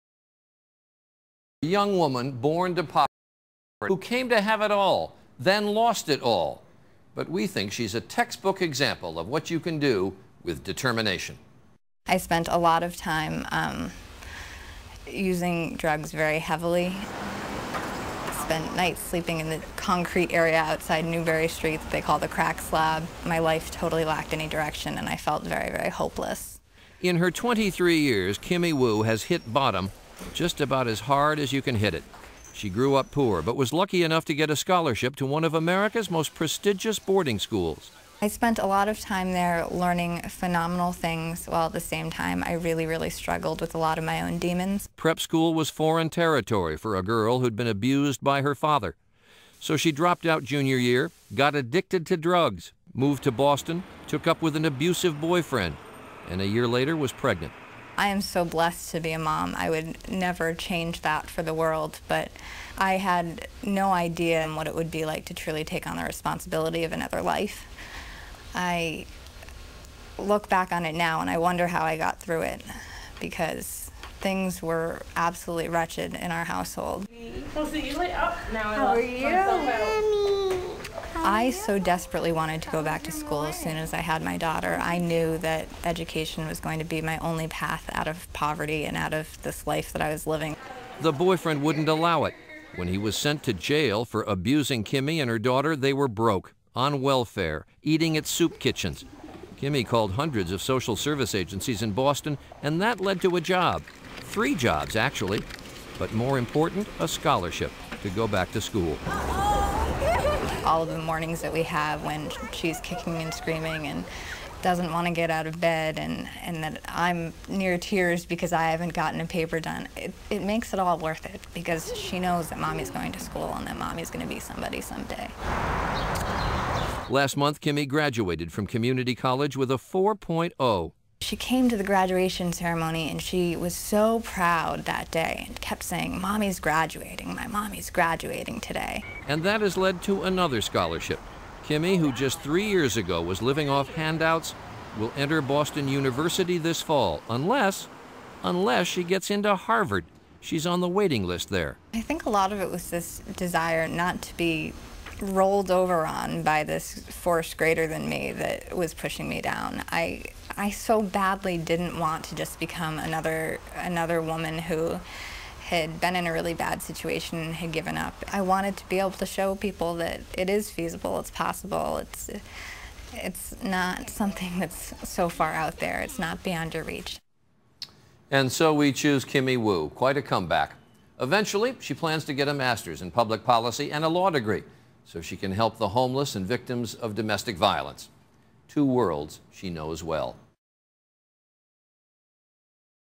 Jack Kent Cooke Foundation Person of the Week
Cooke Scholar Kim Woo's journey from life on the streets to Harvard University is profiled.  Woo is attending Harvard with the support of the Cooke Foundation.